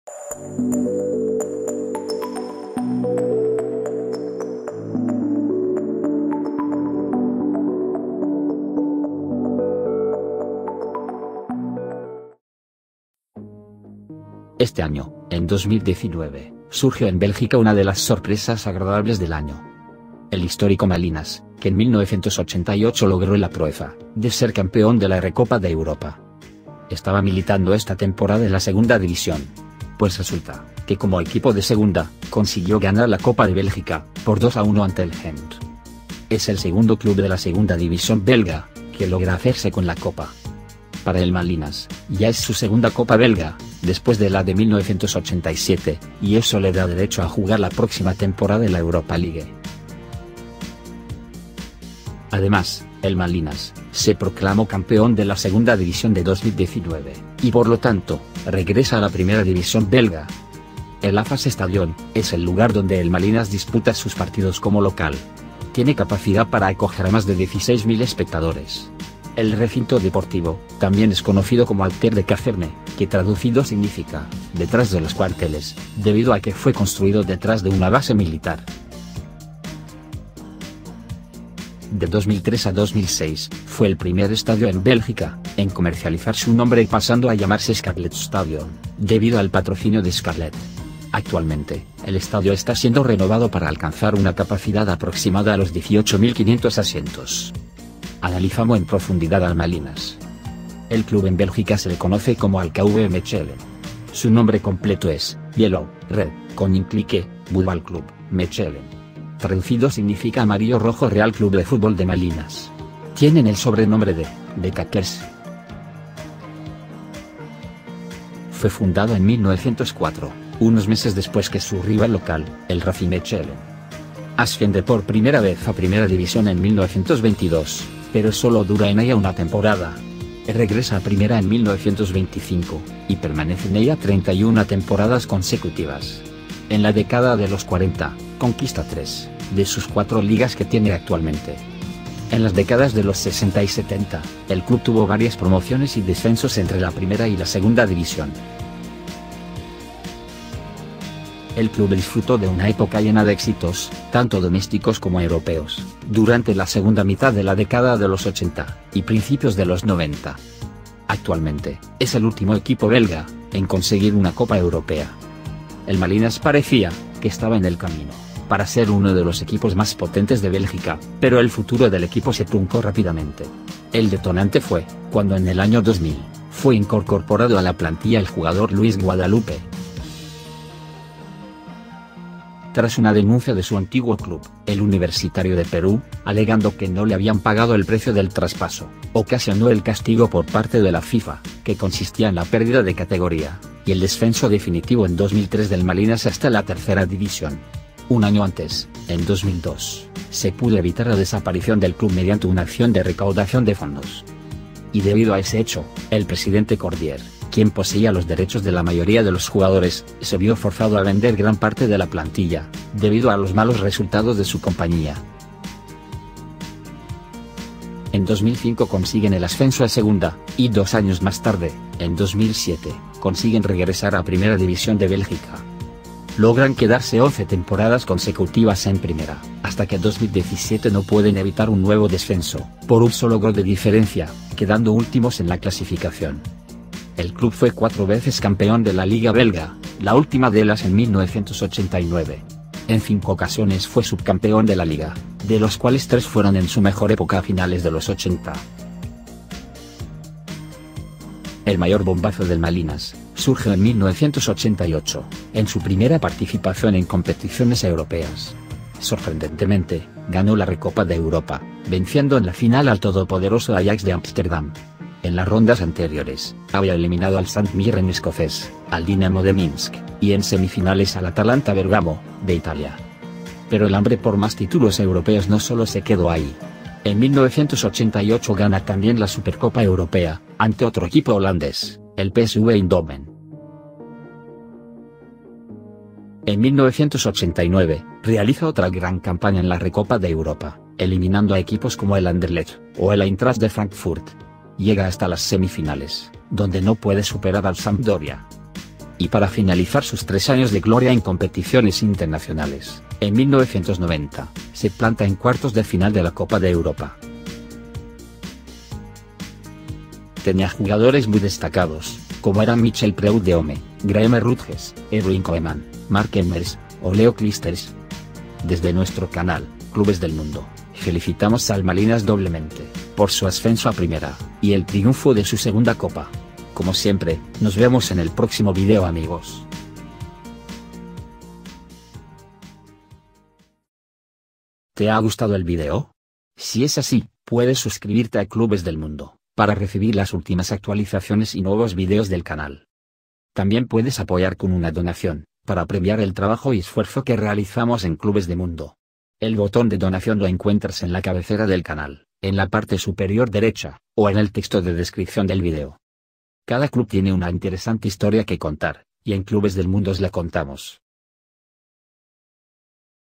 KV MECHELEN - El milagro de Malinas, Campeón Recopa de Europa de 1988 - Clubes del Mundo (Bélgica)
El KV Mechelen, conocido en España como el Malinas, ha sido una de las revelaciones y sorpresas, del futbol nacional continental europeo, del año 2019.

Como equipo de la Segunda División, acaba de ganar la Copa Belga de 2019, ante el Gante por 2-1.  Es la segunda vez que en Belgica, un club de Segunda logre ganar la Copa.
Además, se coronó Campeón de Segunda División, y estara la temporada 2019/20 en la Jupiler League Belga, la maxima categoria.

El Club también es conocido, por haber ganado la Recopa de Europa y la Supercopa de Europa en 1988, siendo hasta la fecha el ultimo club belga en ganar un titulo europeo.
El Malinas cuenta con 4 Ligas y 2 Copas Nacionales ganadas.

Fondo Musical: Audiolibrary Youtube

Si quieres, puedes realizar un Donativo para apoyar el Trabajo del Canal "Clubes del Mundo".